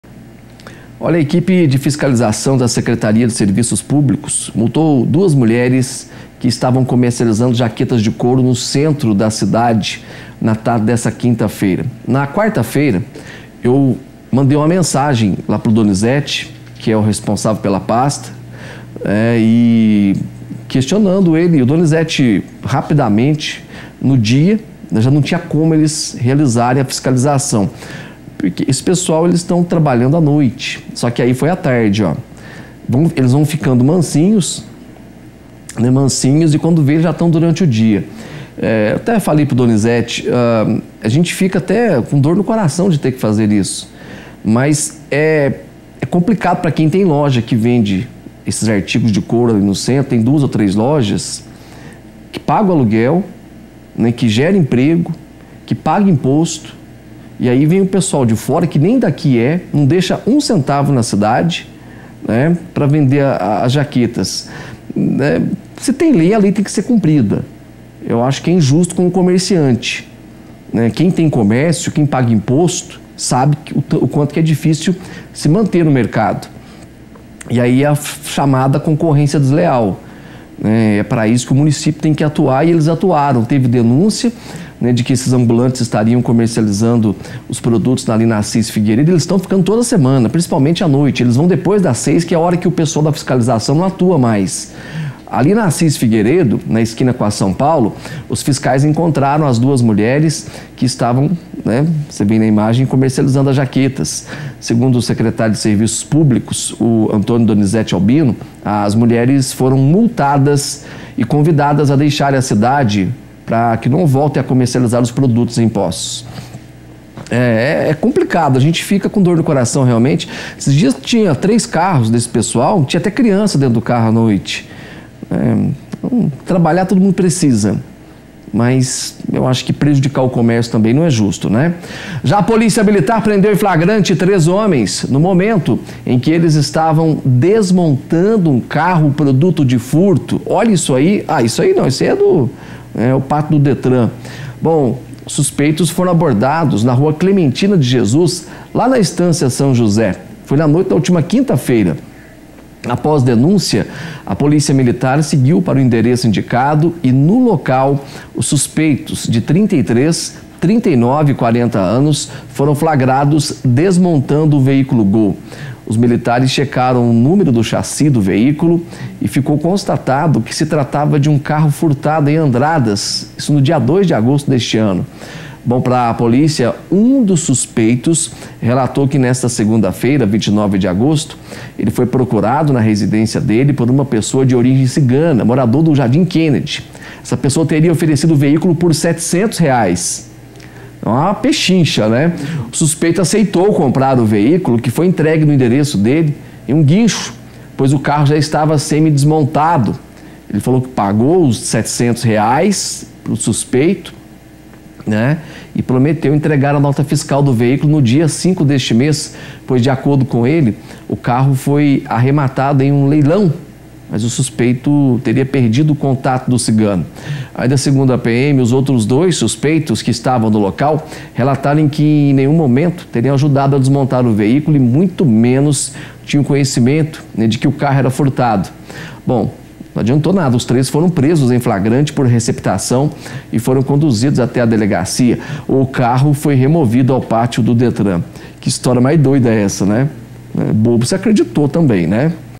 POLÍCIA MILITAR PRENDE EM FLAGRANTE TRÊS HOMENS, NO MOMENTO EM QUE ESTAVAM DESMONTANDO UM CARRO
Os suspeitos foram abordados na rua clementina de jesus, no bairro Estância São José, na noite desta quinta-feira.